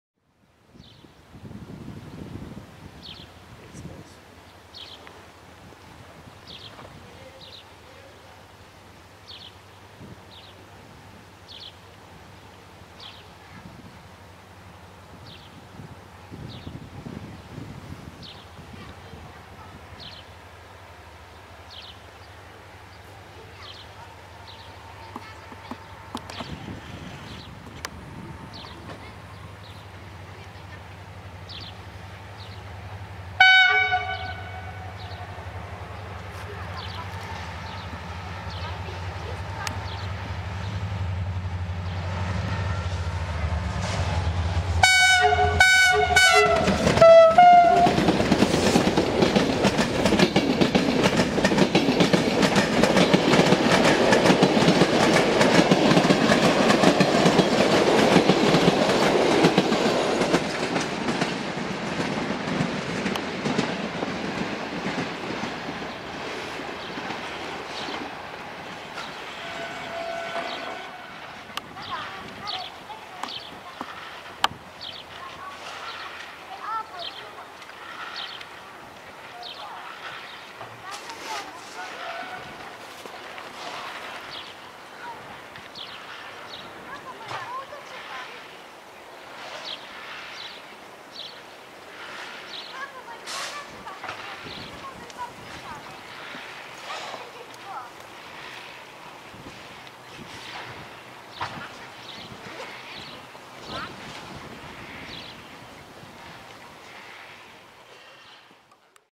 GM Express trece salutand prin Oradea Est (30 06 2012)
RO: LDE2100 EGM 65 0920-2 (aka GM Express) cu IR362 (Cluj Napoca - Budapest Keleti) + IR1833 (Iasi - Timisoara Nord) trece salutand prin Oradea Est

 EN: LDE2100 EGM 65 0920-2 locomotive (aka GM Express) with IR362 (Cluj Napoca - Budapest Keleti) + IR1833 (Iasi - Timisoara Nord) passes saluting through Oradea Est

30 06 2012

(c)19Railfan95